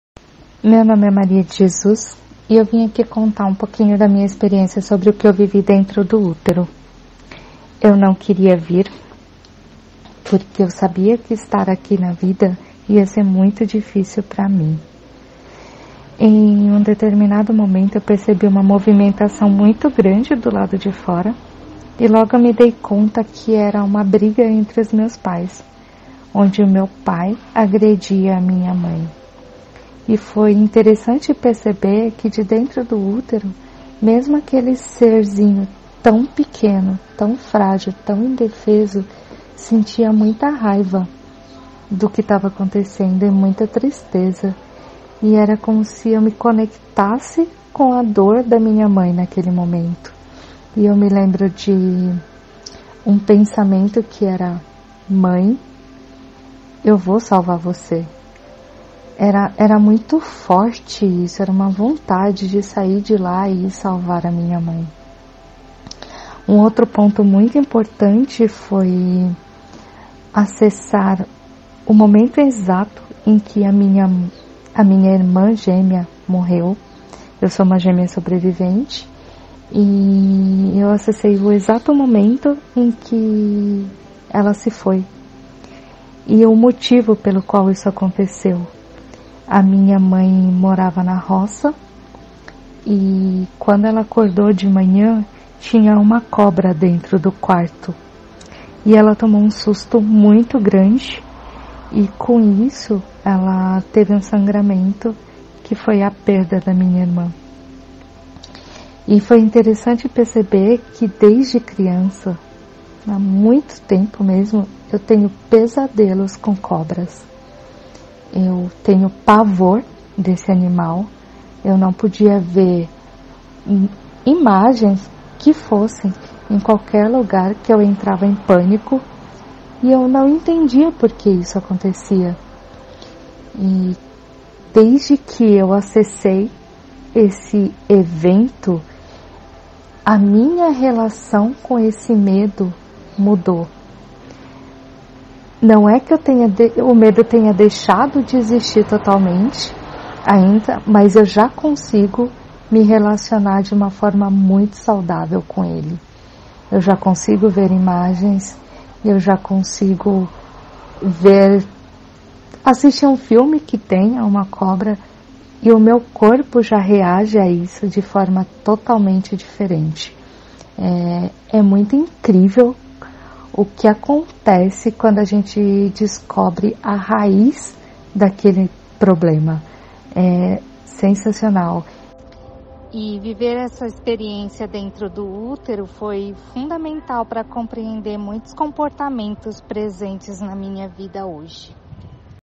DESCOBRI QUE JÁ DENTRO DO ÚTERO EU DECIDI QUE IRIA SALVAR MINHA MÃE | O que vivi no Útero
➡ CURSO ONLINE OS SEGREDOS DA VIDA DENTRO DO ÚTERO

Entre em contato e garanta já sua vaga!